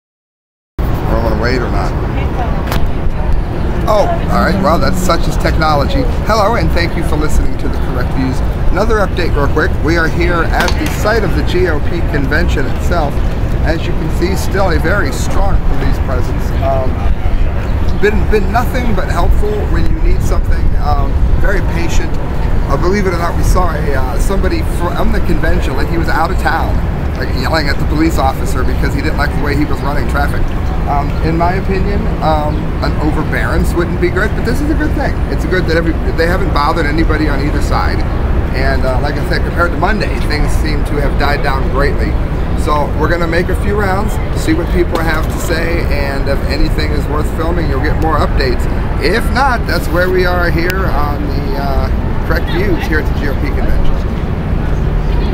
THE CORRECT VIEWS 7 21 2016 Sam Talks GOP Convention Atmophere Mood
'Tis Sam Di Gangi of The Media Speaks with an update on the mood here at the GOP Convention in Cleveland, Ohio. Some police are filmed.